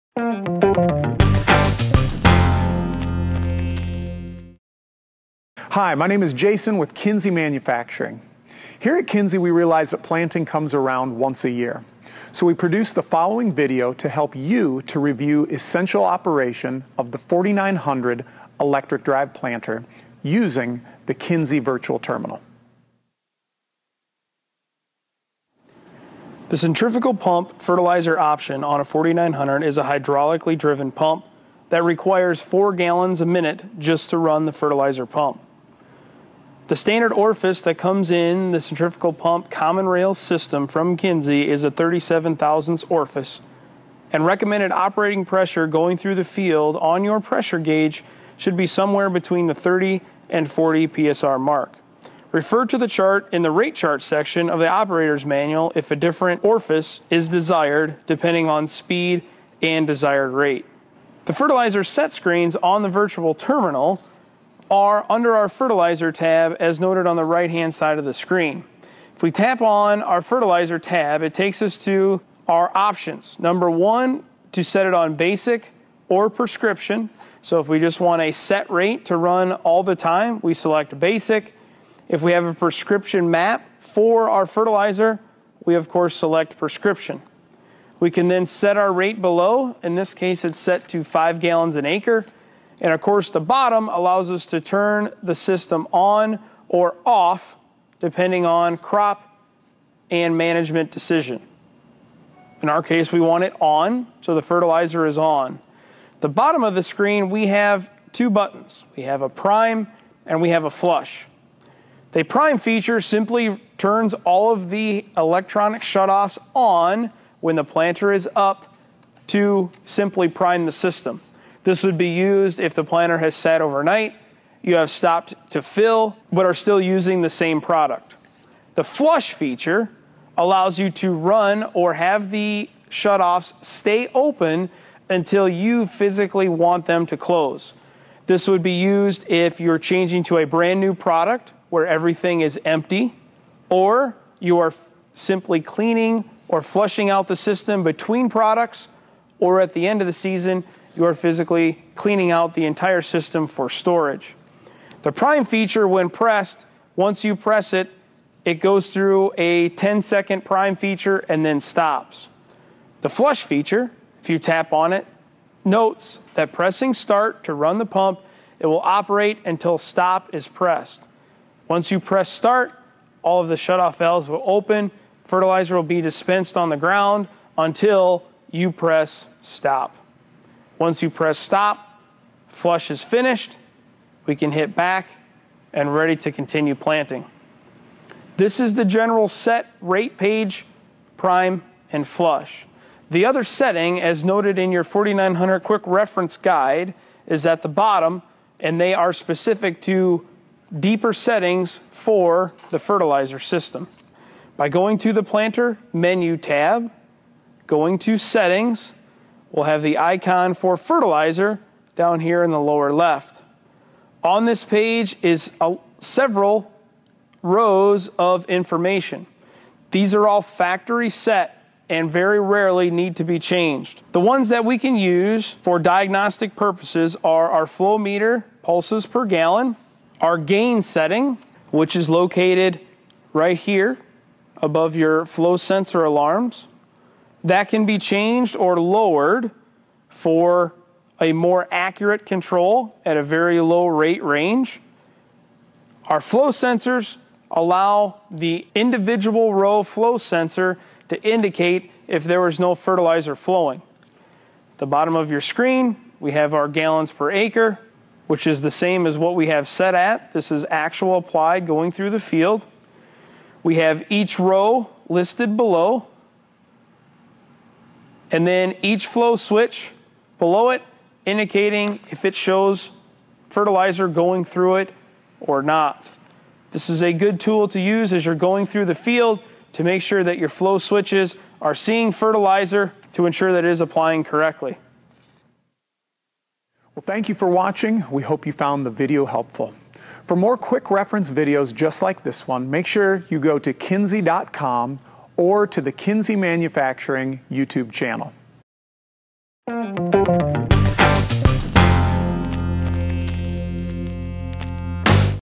Kinze 4900 Startup: 7 - Fertilizer Operation
To prepare for Spring planting with the Kinze 4900 electric drive planter, this video steps through the fertilizer operation features on a Virtual Terminal (VT).